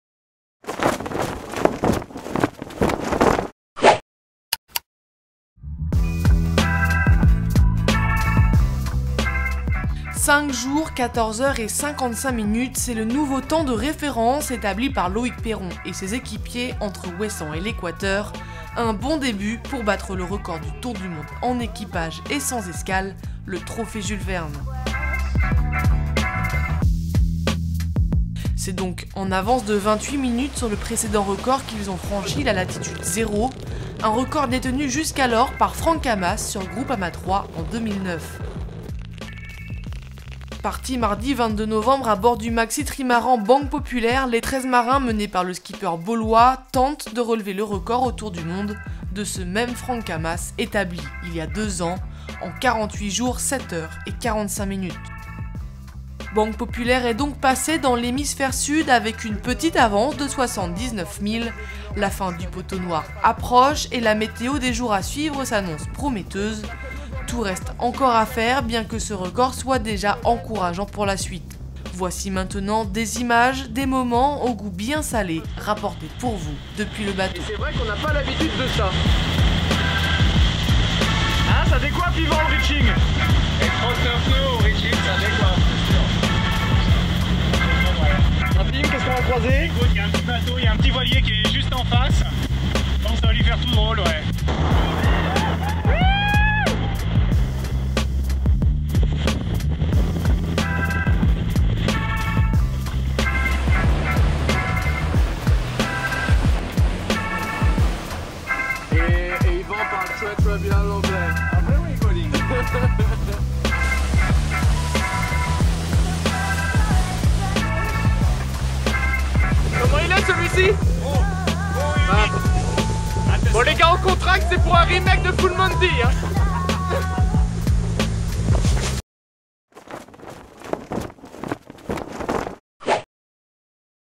La vie à bord de Banque Populaire pendant le record de l'Equateur - Trophée Jules Verne
Lancé dans le Trophée Jules Verne, Banque Populaire V a franchi l'Equateur en établissant un nouveau temps record depuis Ouessant. Le nouveau temps: 5j 14h 55m 10s